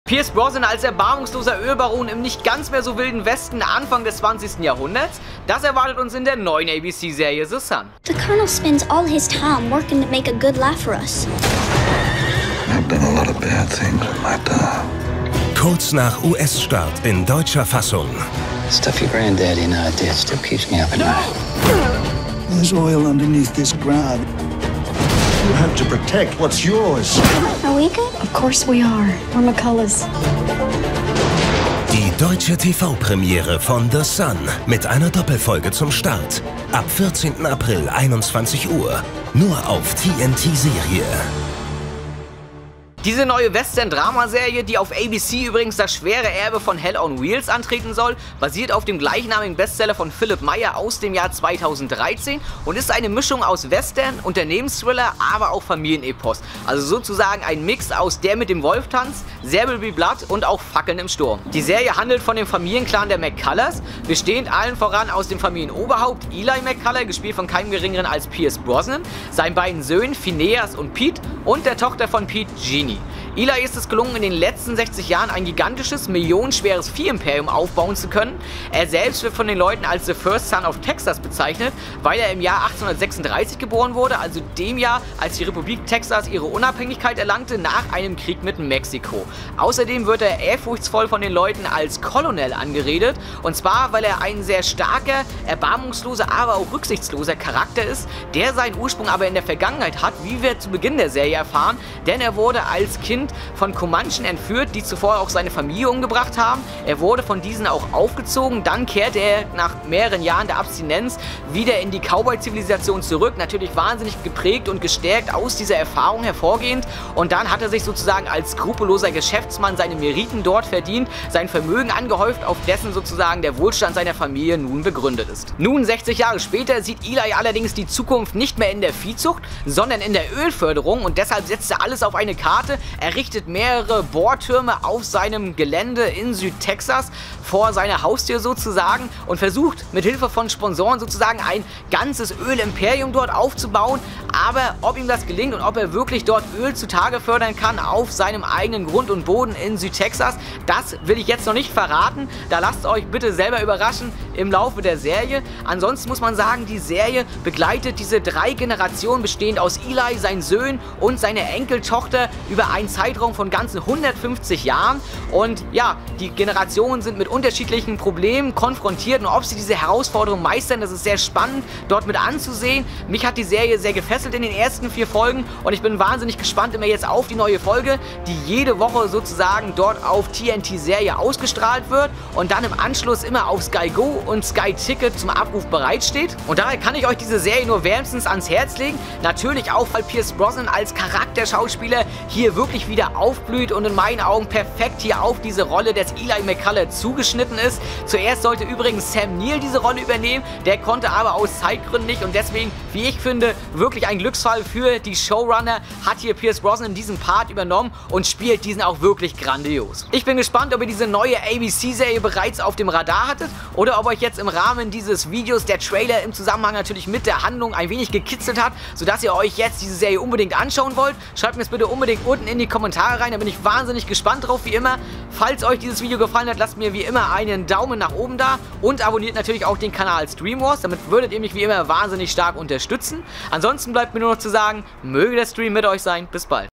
THE SON 🎬Sky Ticket Serie 2017 - Review & Kritik
ＫＥＹ ＦＡＣＴＳ:
🔑 ABC Serie
🔑 Veröffentlichung im deutschen TV auf TNT Serie (Sky)
🔑 Veröffentlichung online ON DEMAND auf Sky Go & Sky Ticket
🔑 Start der 1. Staffel:  14. April 2017
🔑 "Play by Week" - 1 neue Folge pro Woche
🔑 10 Folgen (jeweils ca. 50 Minuten)
🔑 Buchvorlage: "The Son" von Philipp Meyer (2013)
🔑 u.a. mit Pierce Brosnan & Carllos Bardem
🔑 2. Staffel bereits in Planung

ＩＮＨＡＬＴ:
The Son spielt über einen Zeitraum von 150 Jahren und begleitet die Familie McCollough und ihr Ölunternehmen. Die 1. Staffel beginnt mit dem Familienpatriarchen Eli McCullough (Pierce Brosnan), der das seit mehreren Generationen in der Hand der McCulloghs liegende Ölunternehmen leitet, doch er erleidet eine große Niederlage. Langsam lernt er, die Rohstoffe der Natur effizienter zu nutzen und beginnt, ein gigantisches Ölimperium aufzubauen. Doch sein Weg zum Erfolg ist mit zahlreichen Opfern und neuen Feinden gepflastert und auch die Konkurrenz schläft nicht. Doch Eli ist kein üblicher Ölbaron: Als Kind würde er von Ureinwohnern Amerikas entführt, wuchs bei ihnen auf und hat auch heute noch eine große Verbundenheit zu dem Häuptling Toshaway (Zahn McClarnon)

ＣＡＳＴ:
❌Pierce Brosnan
❌Henry Garrett
❌Paola Nuñez
❌Carlos Bardem

ＤＲＥＨＢＵＣＨ:
❌Brian McGreevy
❌Philipp Meyer
❌Lee Shipman

ＲＥＧＩＥ:
❌Kevin Dowling (2 Episoden)
❌Olatunde Osunsanmi (2 Episoden)
❌Jeremy Webb (2 Episoden)
❌John David Coles  (1 Episode)
❌Tom Harper (1 Episode)

📓 Den Bestseller DER ERSTE SOHN (THE SON) von Philipp Meyer bekommt ihr

📺 Noch kein SKY Ticket? Dann gehts hier zum kostenlosen Probe-Monat (jederzeit kündbar)